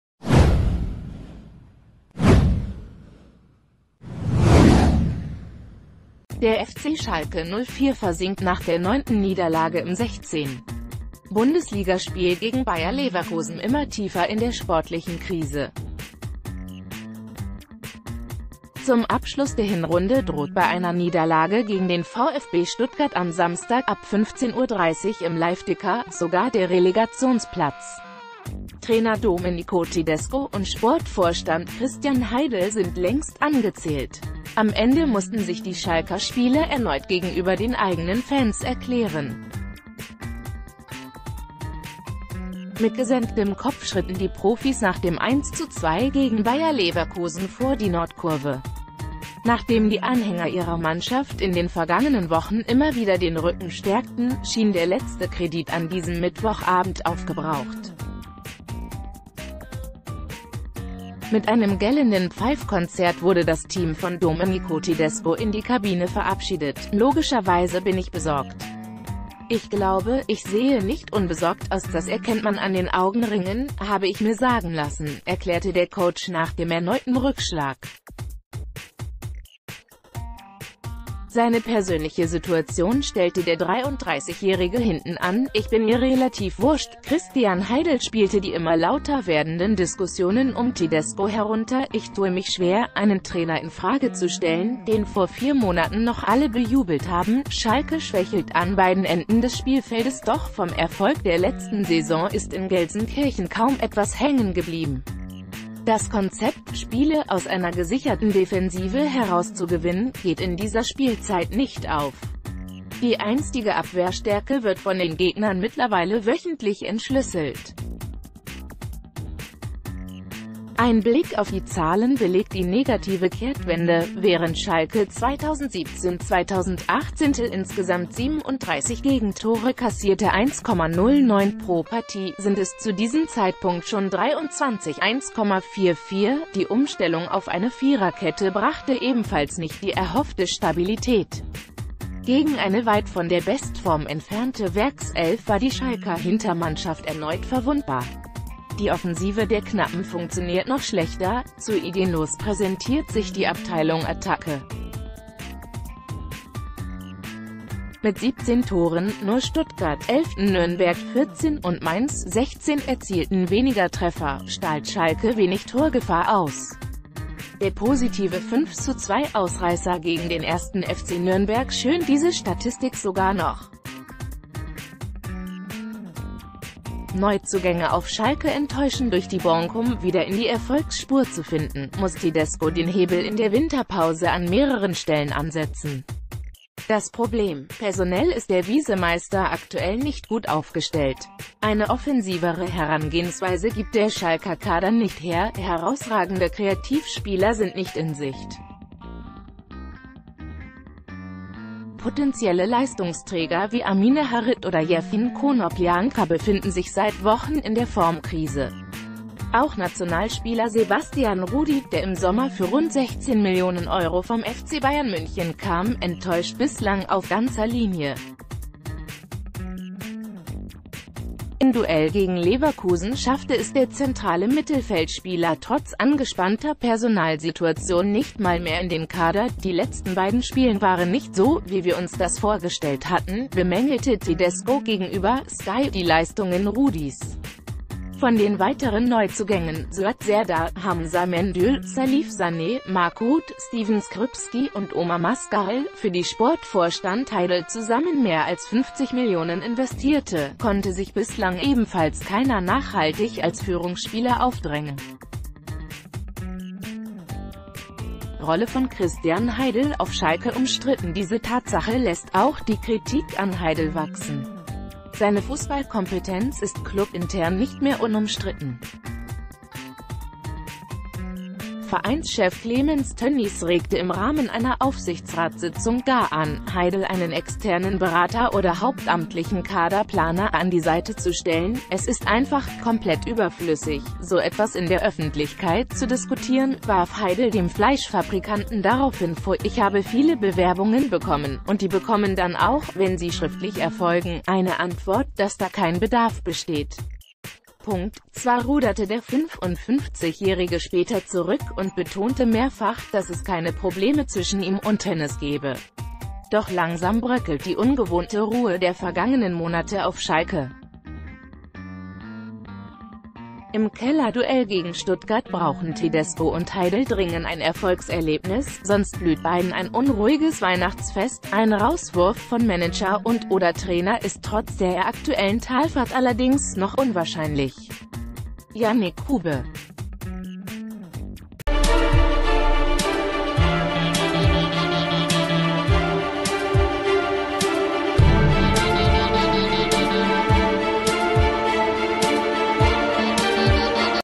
Darum rutscht Schalke immer tiefer in die Krise
Der FC Schalke 04 versinkt nach der neunten Niederlage im 16. Bundesliga-Spiel gegen Bayer Leverkusen immer tiefer in der sportlichen Krise. Zum Abschluss der Hinrunde droht bei einer Niederlage gegen den VfB Stuttgart am Samstag (ab 15:30 Uhr im Live-Ticker) sogar der Relegationsplatz. Trainer Domenico Tedesco und Sportvorstand Christian Heidel sind längst angezählt. Am Ende mussten sich die Schalker Spieler erneut gegenüber den eigenen Fans erklären. Mit gesenktem Kopf schritten die Profis nac...